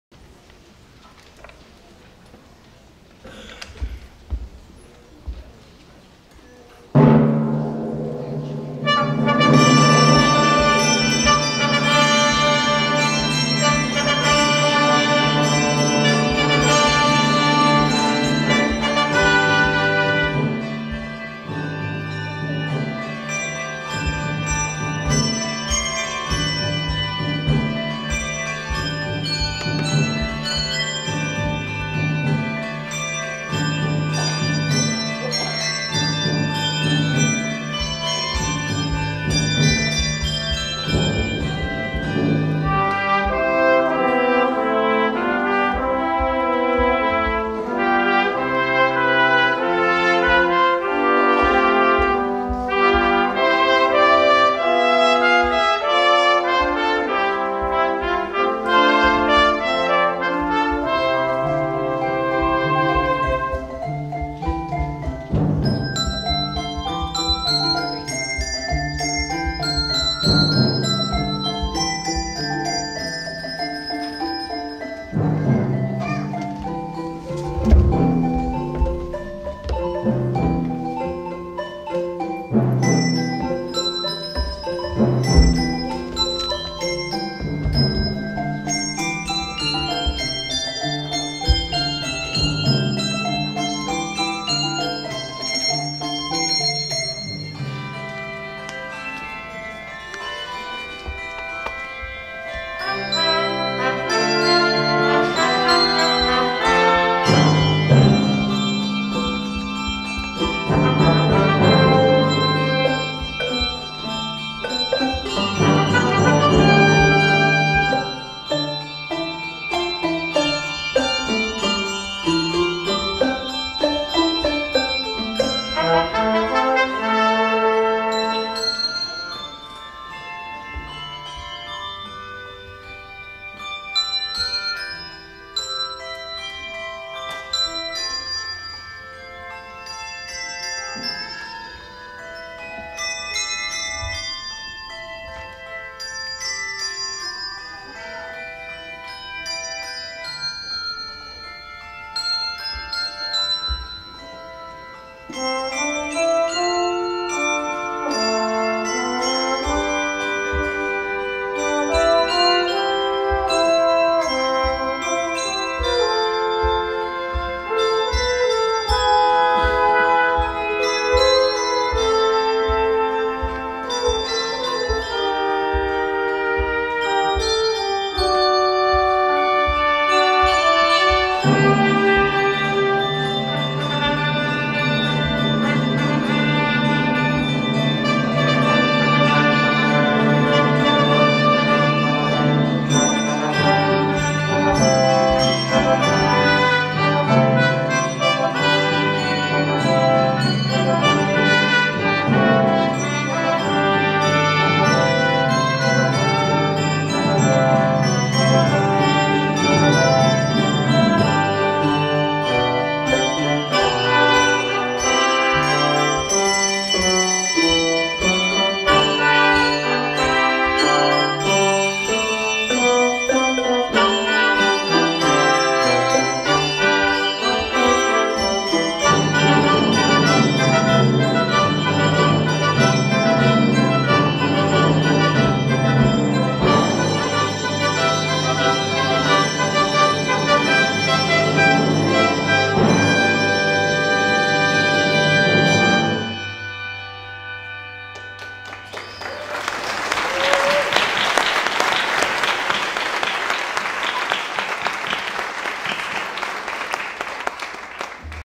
Shephills Easter Bell Choir w/brass Quartet and Timpani!
Kevin McClure, Shephills Director of Worship & Arts and organist extraordinaire, conducts the Shephills Bell Choir with brass quartet led by Robert Cannon, Principal Trumpet of Austin Symphony along with Timpani, played by Tony Edwards, Principal Timpanist of Austin Symphony on Easter Sunday, April 1, 2018!